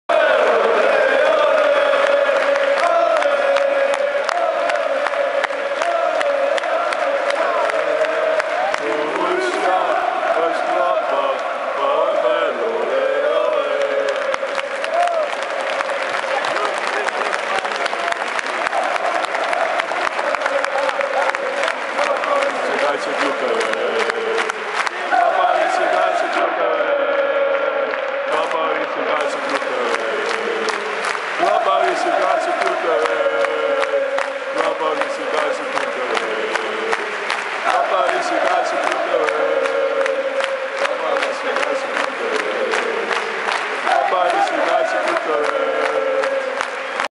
Lazio-Gladbach
Na uur nog altijd vastgehouden in het uitvak, spelers komen nogmaals bedanken